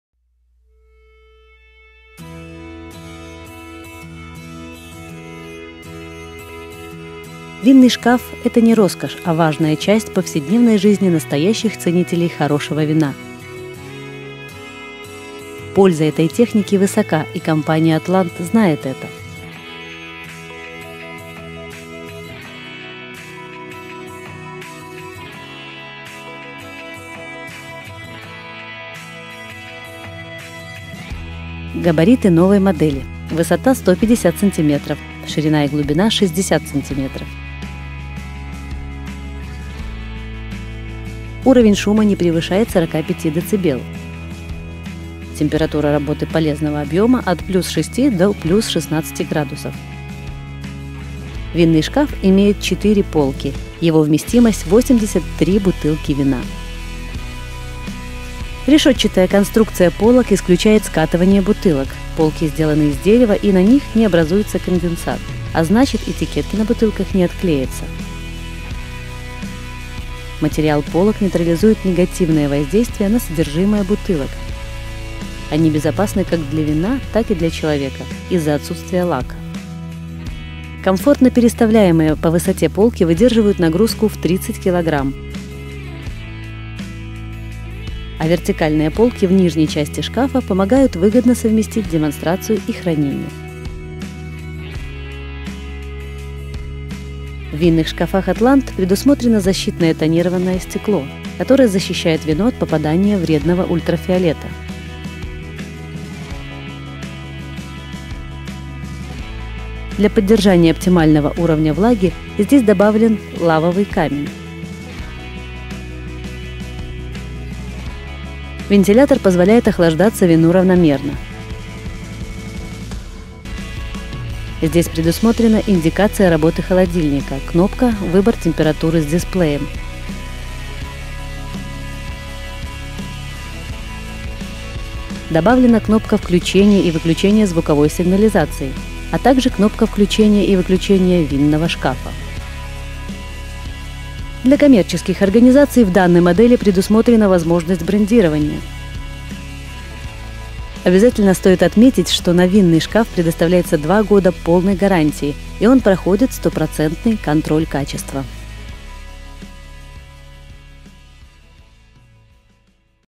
Винный холодильник ATLANT ХТ-1008. Обзор винного холодильника с увеличенном объемом
Обзор торгового однокамерного холодильника ATLANT ХТ-1008.
Винные холодильники ATLANT — торговое оборудование, без которого нельзя представить успешную работу современного ресторана, бара, кафе. Для заведений общественного питания, которые предлагают своим посетителям широкий ассортимент различных вин, винные холодильники особенно необходимы. Не будут лишними винные холодильники и в домашнем хозяйстве ценителей винодельческого искусства. Эти холодильники создают особые условия для хранения вина, которые идентичны условиям хранения в винном погребе. Все эти возможности имеют торговые холодильники «АTLANT» ХТ-1007, ХТ-1008, предназначенные для охлаждения, хранения и демонстрации вина в стеклянных бутылках.